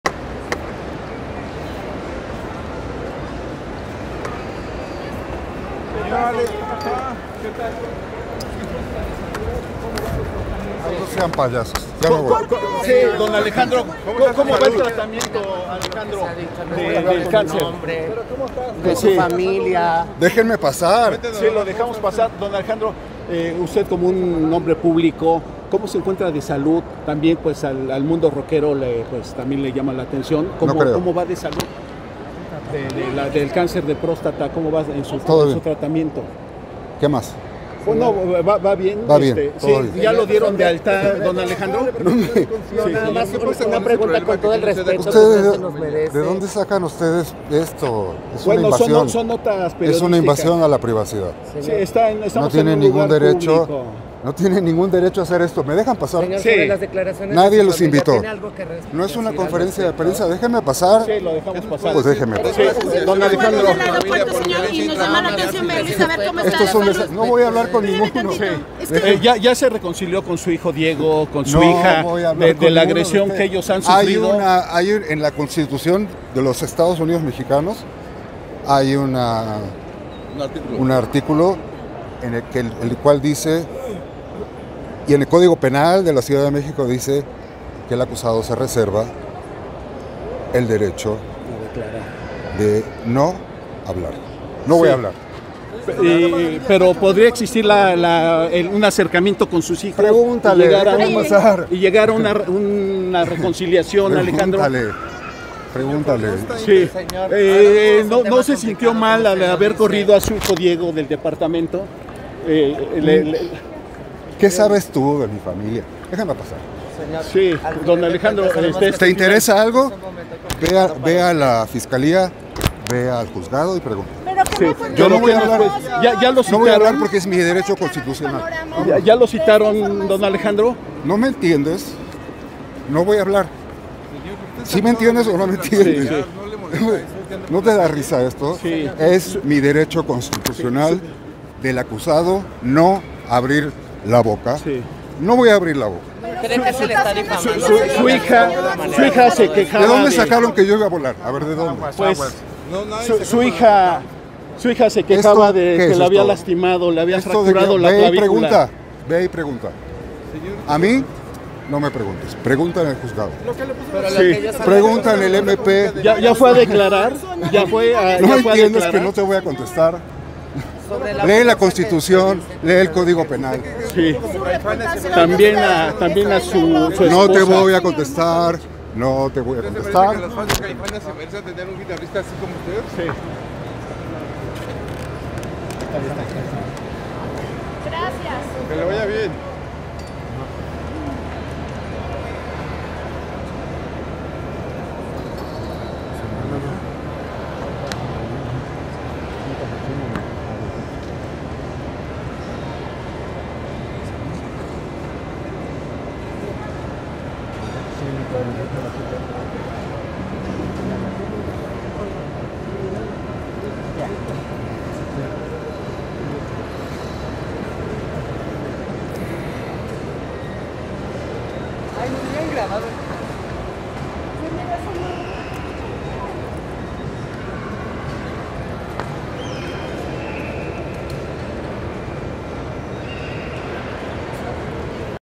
"Ustedes qué saben": Alejandro Marcovich ex guitarrista de Caifanes golpeó a sus hijos y esposa
El ex guitarrista de Caifanes está enfermo de la próstata corrió a su hijo Diego de su casa por quedarse dormido en una banca afuera de su edificio luego de padecer una enfermedad, a su hija le rompió la clavícula y tiene demanda familiar.